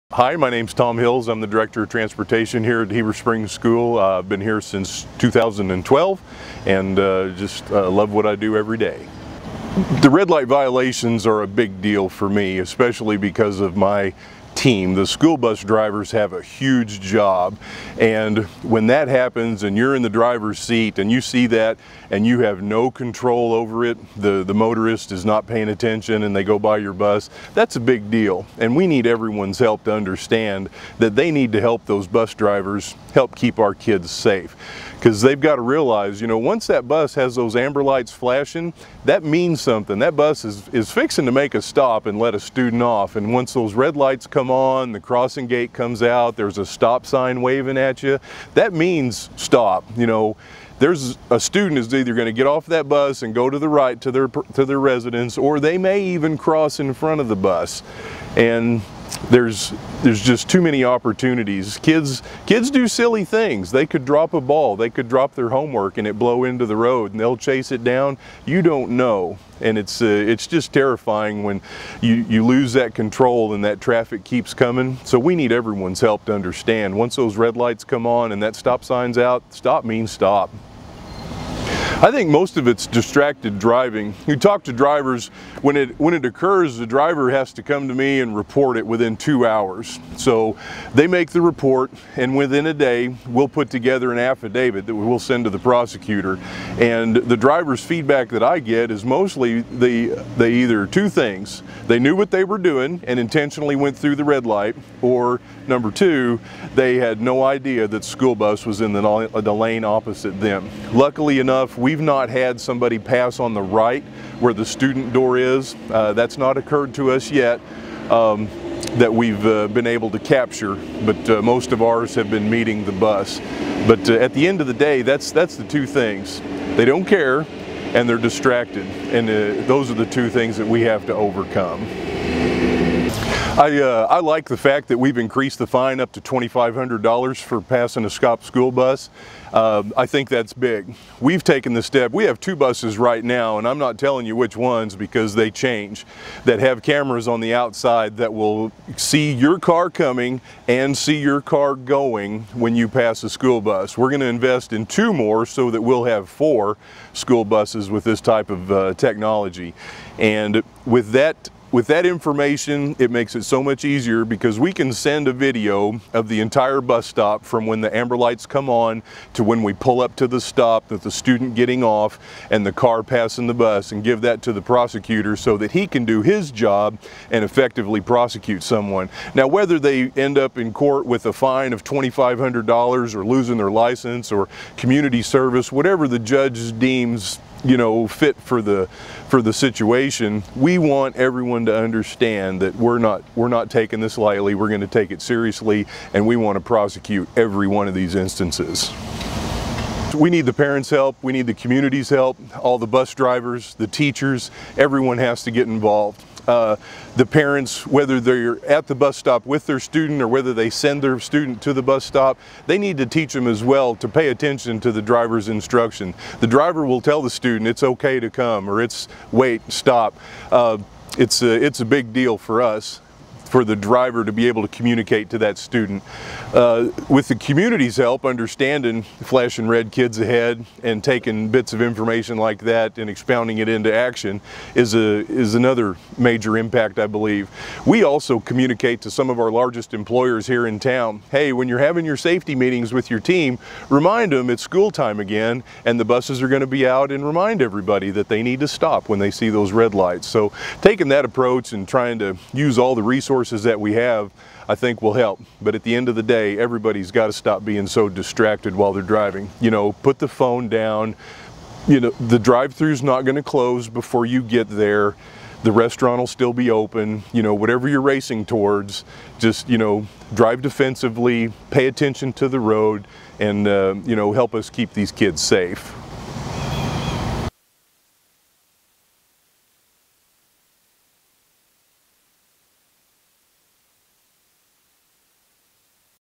Flashing Red. Kids Ahead Campaign - Heber Springs School District (Tom Hills)
Heber Springs School District Transportation Director Tom Hills shared his message on school bus safety as a part of the Flashing Red. Kids Ahead. campaign.